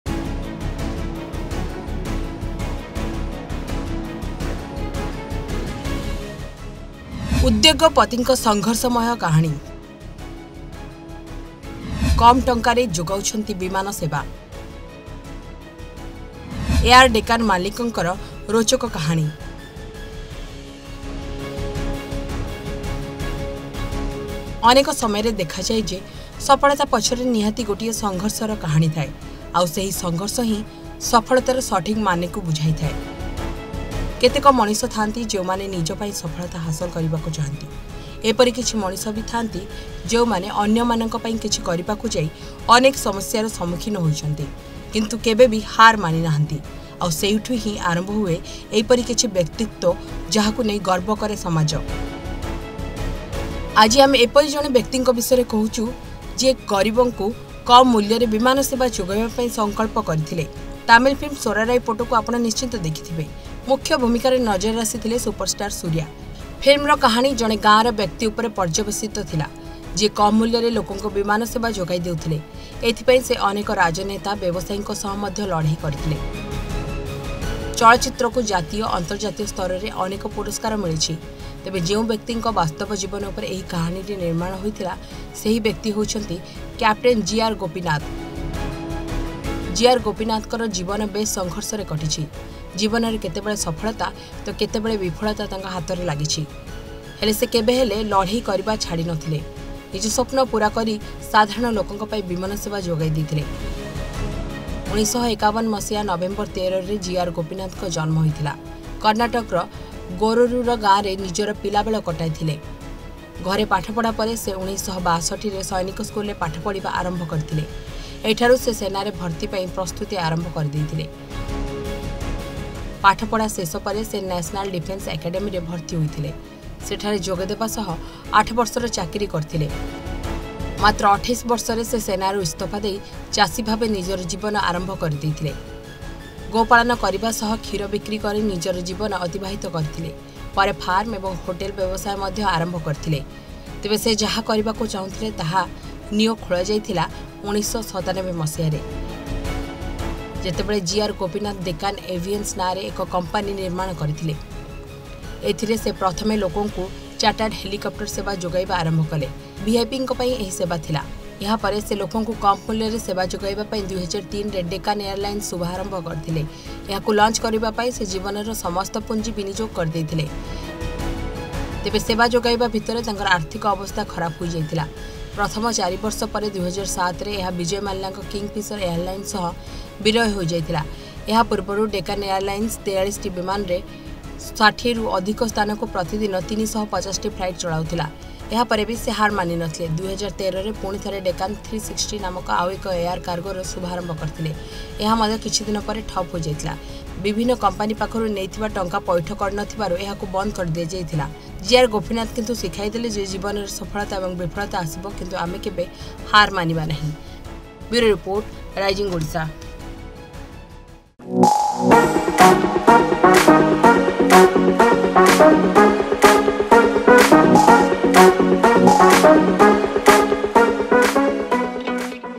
Farmer to Airlines Owner: 'Air Deccan' Founder GR Gopinath, Who Made Flying Affordable to Millions
Watch the special program on Air Deccan Founder GR Gopinath Struggle Story.